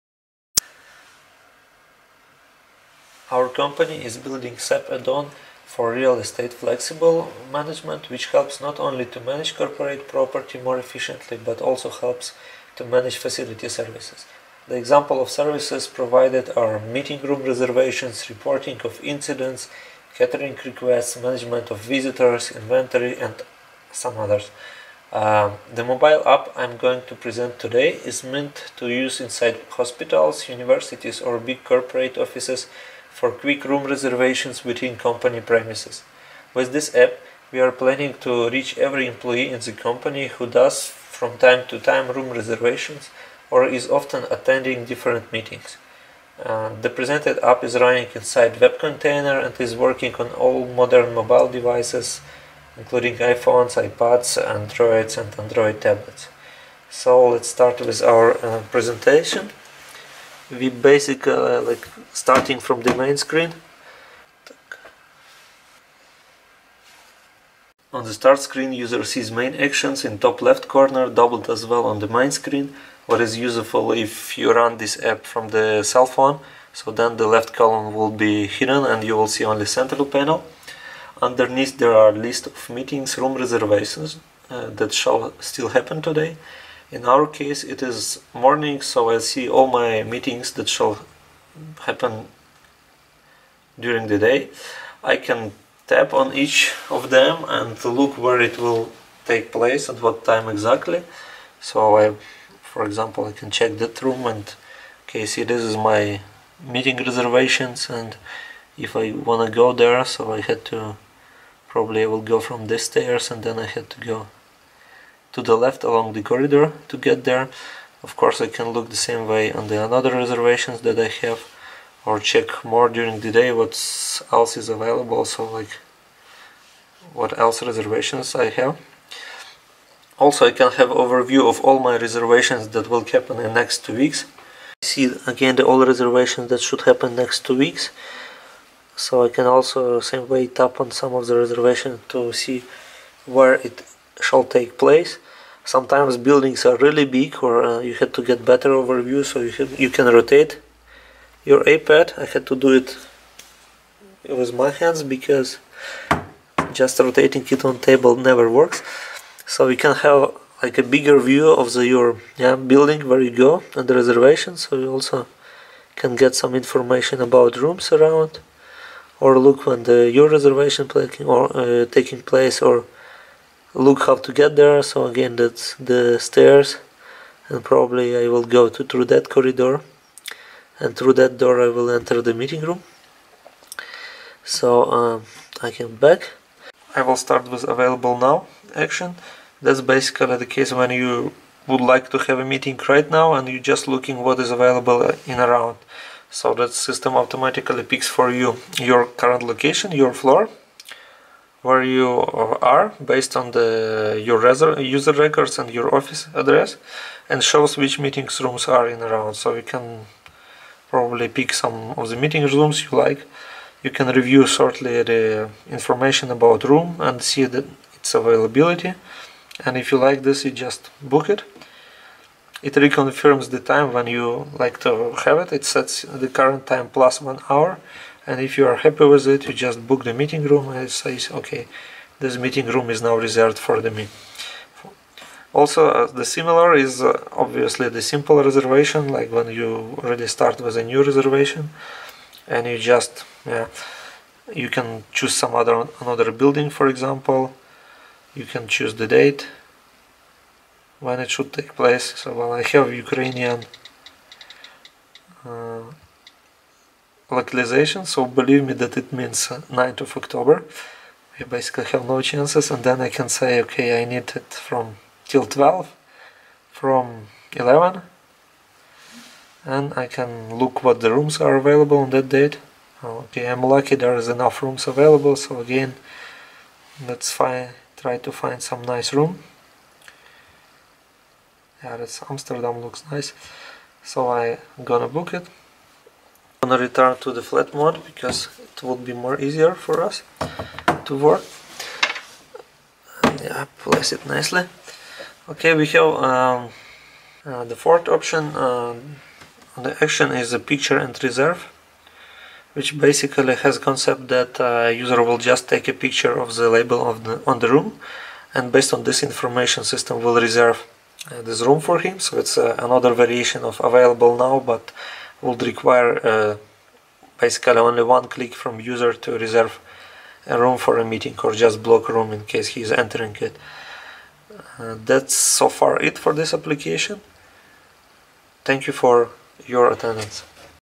mReservation - mobile app for room reservations with SAP backend using RE-FX module
mReservation is aimed to increase productivity at corporate offices, hospitals and universities by providing easy and intuitive way to reserve/book rooms by company employees.

As it allows for every employee in the company who does require, from time to time, any kind of room reservation or often attending different meetings. The app is running inside web container and is working on all modern mobile devices like iPhone, iPad, Android and Android tablets or can be called directly from internet leaving zero-footprint on mobile devices.

Using SAP Real Estate Flexible (RE-FX) on the back end with our own add on this app helps to manage more efficiently available facilities by providing quick and intuitive way of meeting room reservations, catering requests and management of visitors